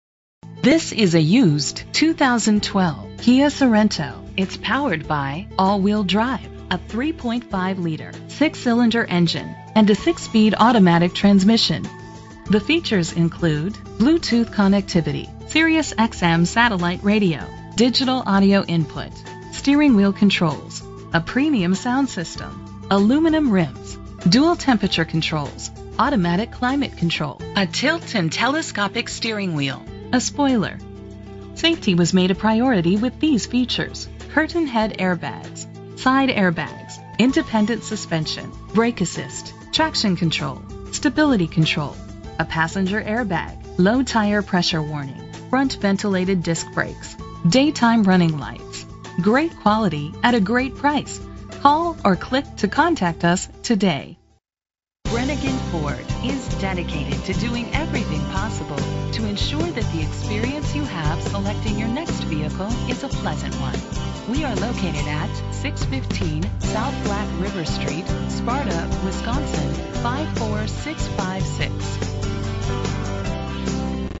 2012 Kia Sorento - Sparta WI
Phone:
Year: 2012
Make: Kia
Model: Sorento
Trim: EX
Engine: 3.5 liter 6 cylinder 24 valve
Transmission: 6-Speed Automatic
Color: Java
Mileage: 64568
Address:
615 South Black River Street